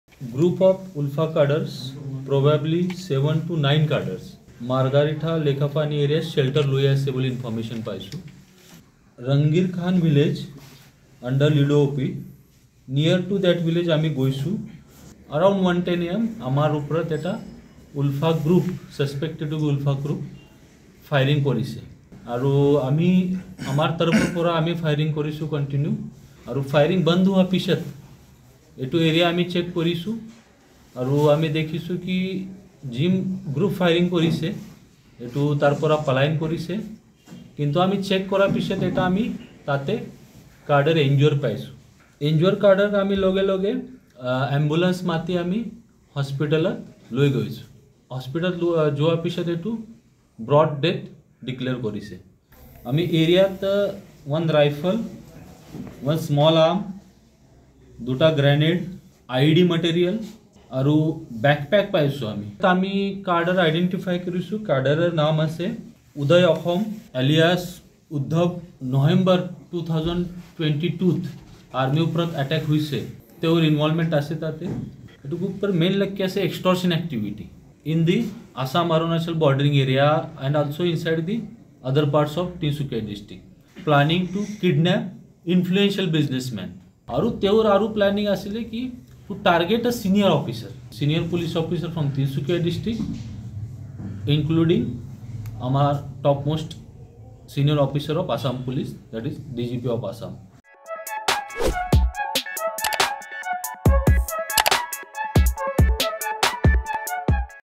ULFA-I cadre killed in Tinsukia planned to target Assam DGP
The ULFA-Independent cadre who was killed in an encounter with Assam police in the wee hours today had made plans to target the state DGP and senior Police officers in Tinsukia. This was revealed by Tinsukia SP, Abhijeet Gurav.

SP Gurav also informed that around 7-9 suspected ULFA-Independent cadres were involved in today's encounter in which the outfit's top self-styled field commander Uday Axom was killed.